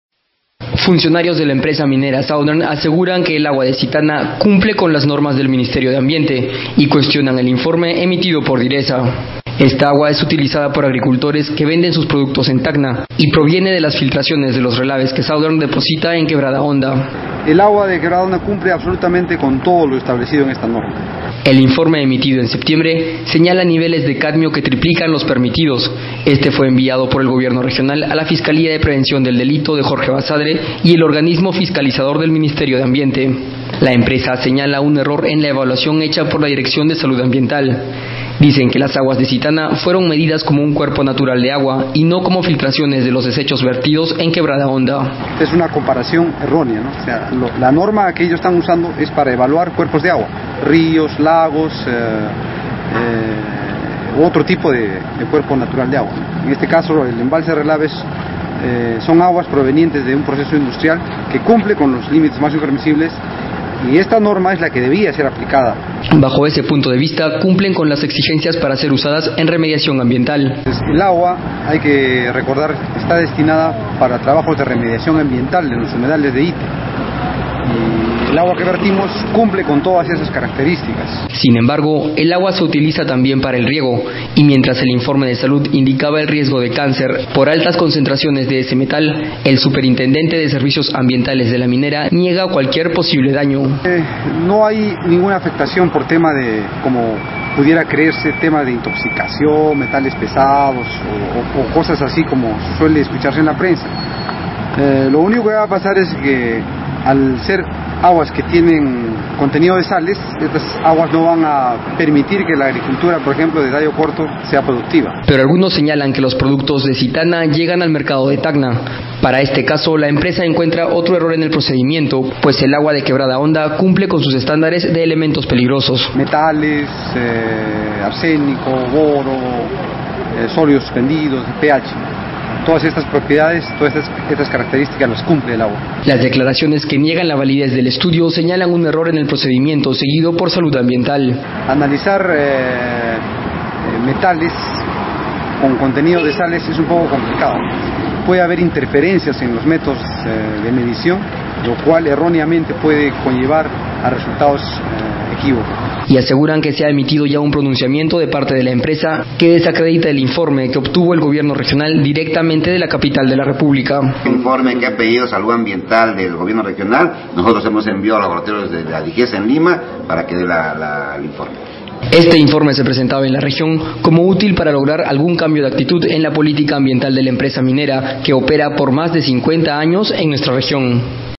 SOUTHERN NIEGA CONTAMINACIÓN EN SITANA
Funcionarios de la empresa  minera Southern aseguran que el agua de Sitana, cumple con las normas del ministerio de ambiente  y cuestionan el informe emitido por DIRESA.  Esta agua es utilizada por agricultores que venden sus productos en Tacna.  Y proviene de las filtraciones de los relaves que Southern deposita en Quebrada Honda.